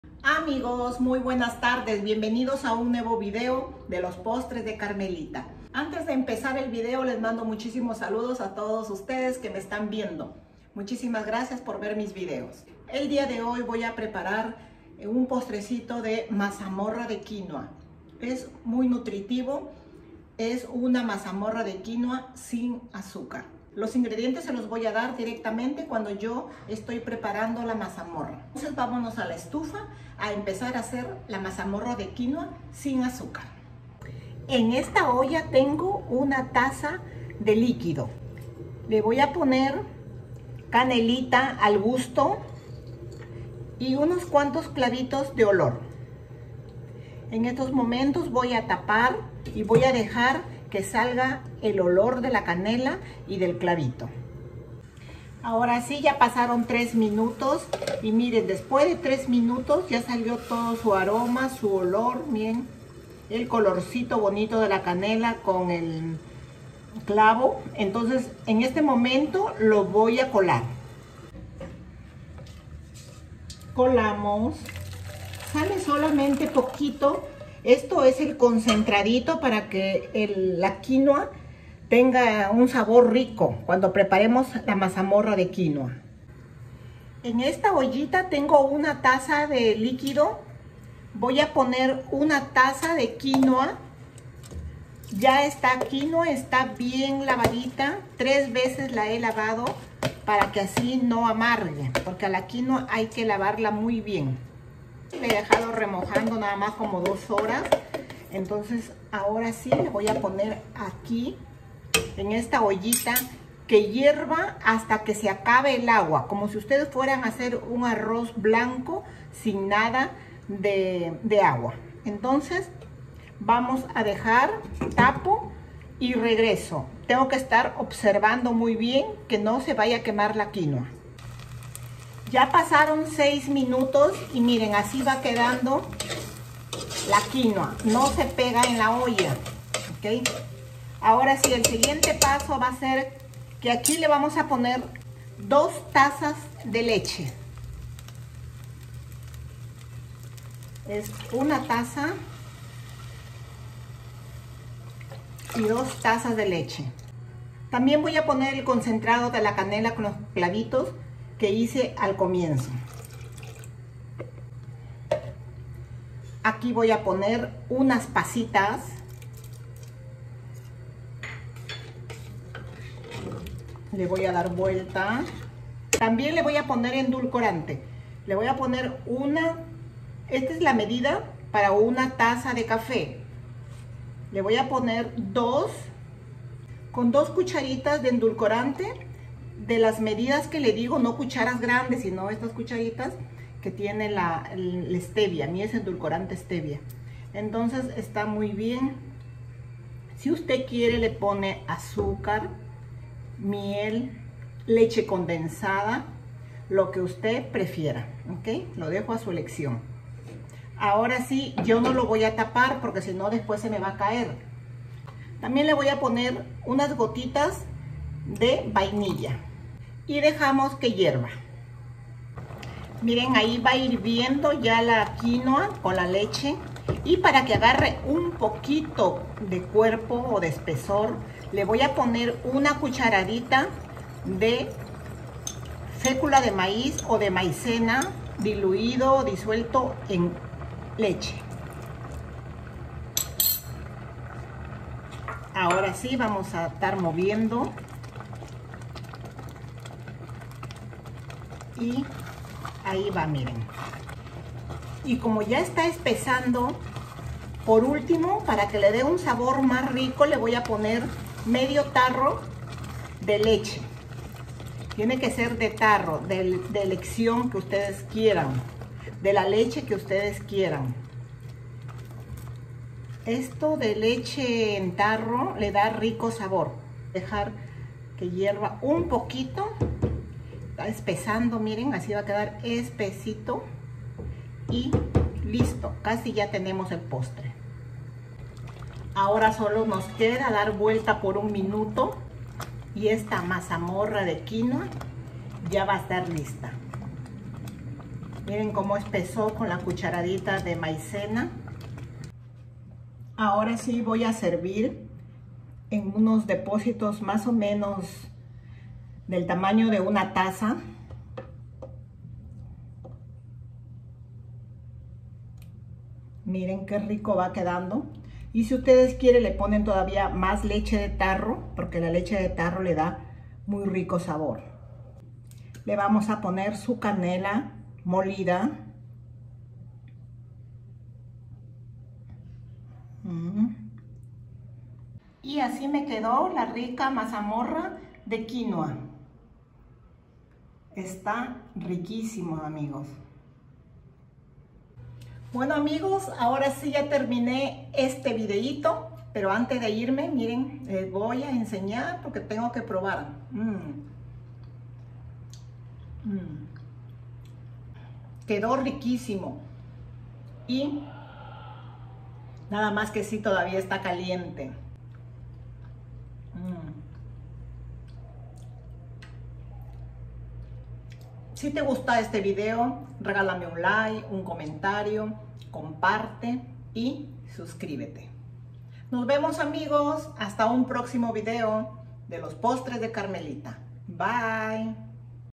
RICA MAZAMORRA DE QUINUA SIN AZÚCAR postres quinua mazamorra.
Amigos bienvenidos a un nuevo videos de los postres de Carmelita.el día de hoy les prepare una rica mazamorra de quinua sin azúcar muy nutritiva deseo que lo prepares y que te guste.

Ingredientes

1 taza de quinua bien lavada y remojada por 2 horas
2 tazas de leche de vaca
1/2 taza de leche evaporada de tarro
Palito de canela
5 clavos de olor
2 tazas de líquido
Endulcorante estevia
Pasas al gusto